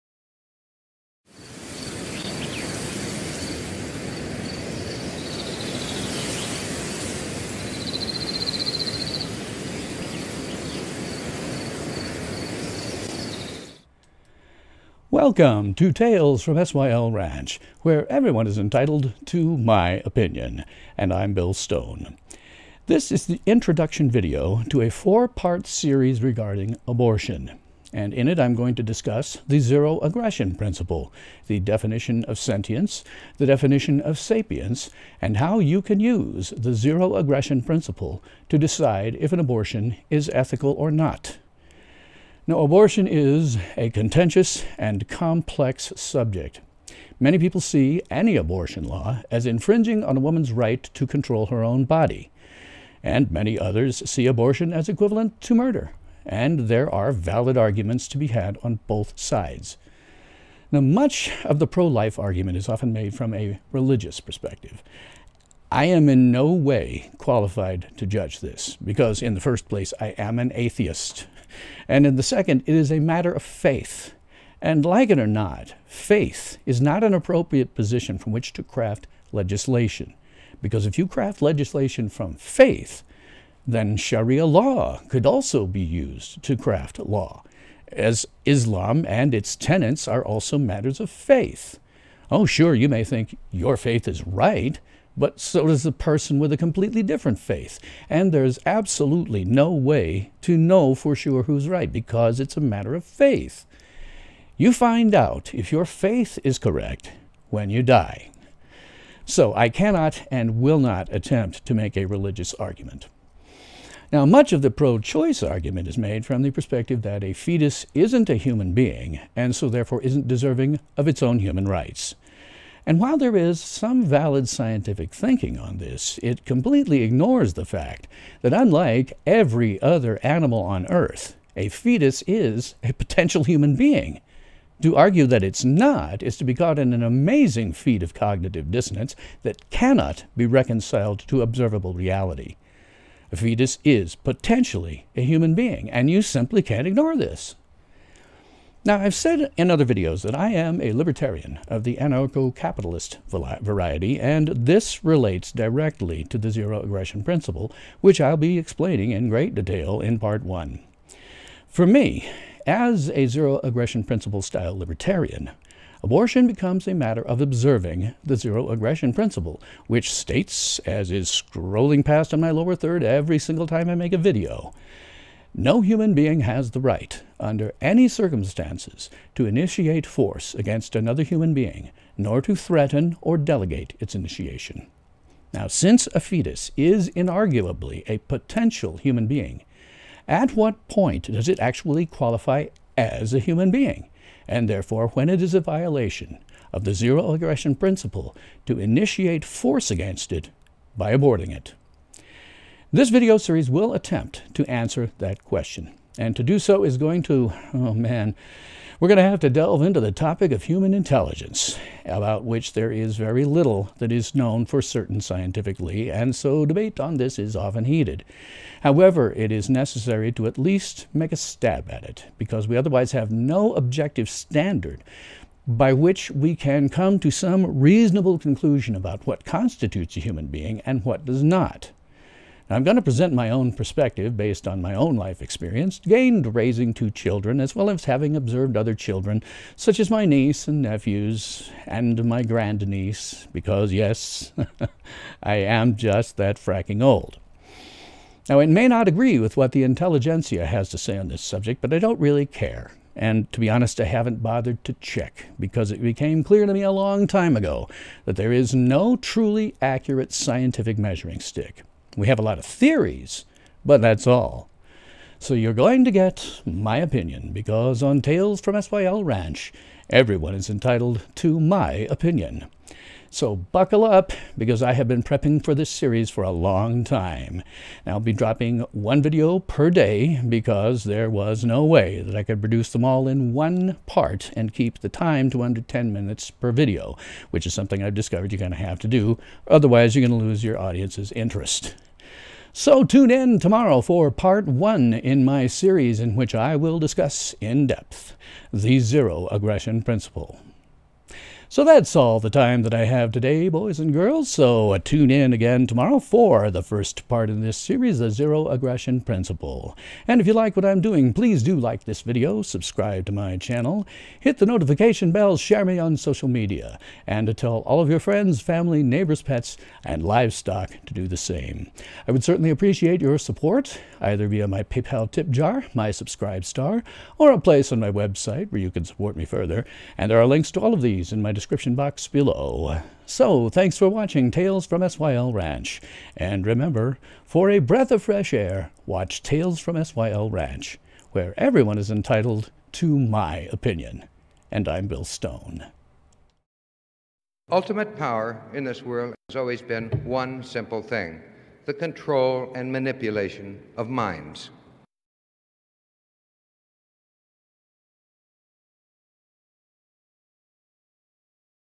Abortion and the ZAP: Introduction
This episode is the introduction to a 4-part series on abortion and the Zero Aggression Principle.  Part 1 will discuss the ZAP.  Part 2 will discuss sapience.  Part 3 will discuss sentience.  Part 4 will explain when the ZAP applies to a fetus.